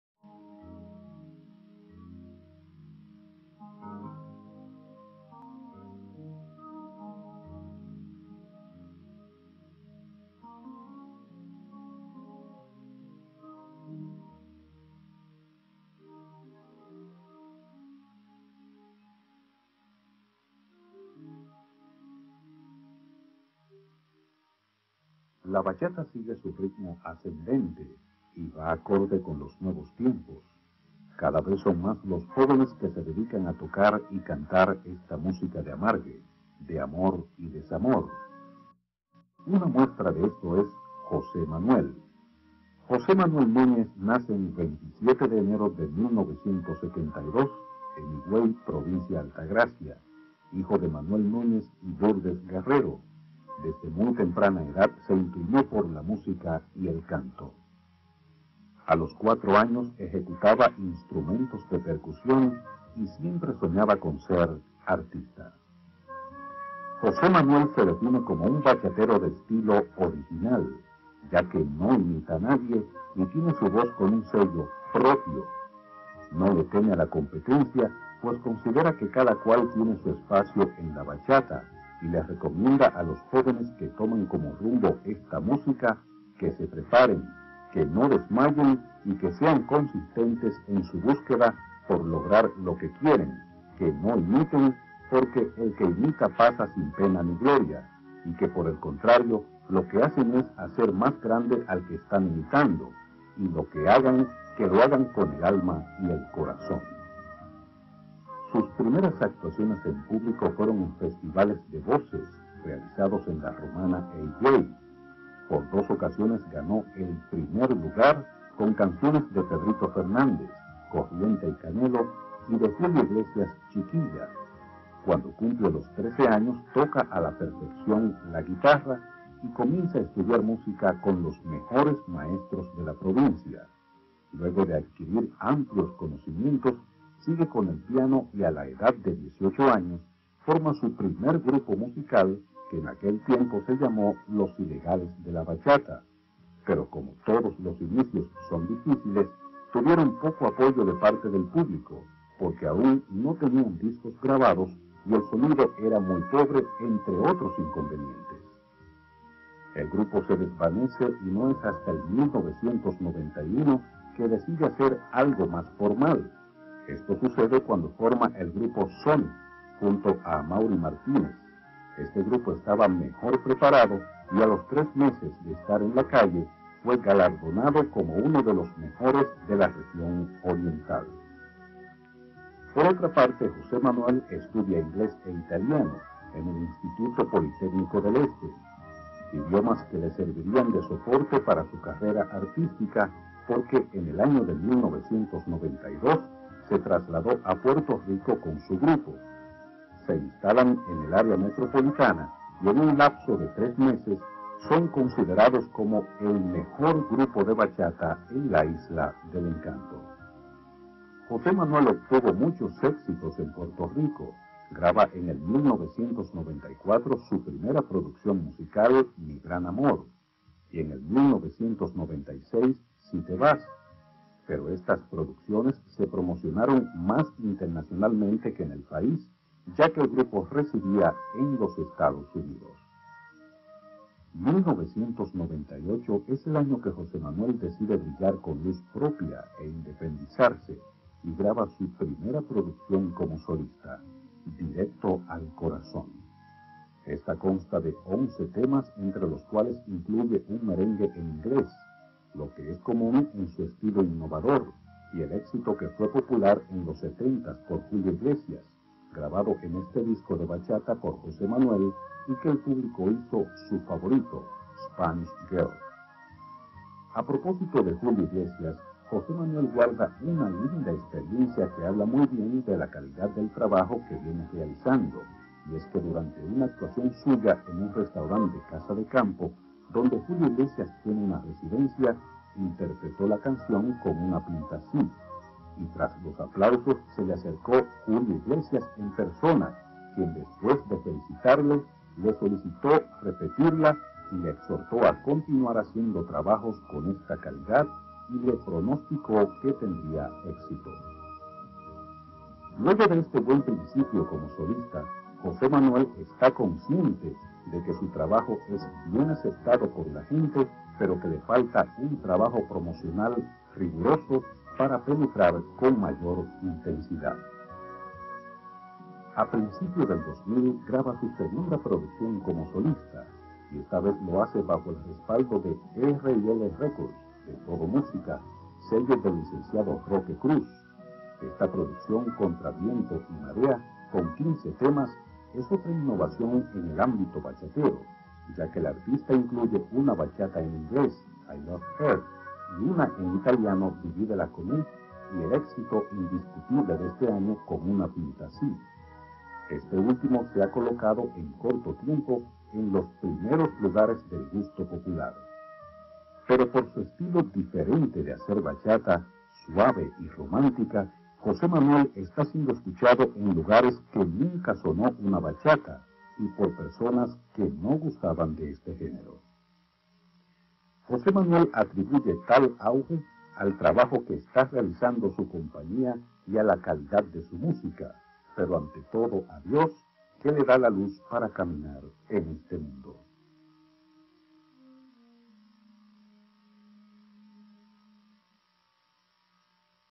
Jose Manuel El Sultan Vida y trayectoria hasta 2000
DOCUMENTAL DESDE SU NINEZ  HASTA ANO 2000  DEL SULTAN  GRABADO EN BOCA DE YUMA SU SITIO NATAL la mejor bachata romantica del mundo el artista de bachata mas completo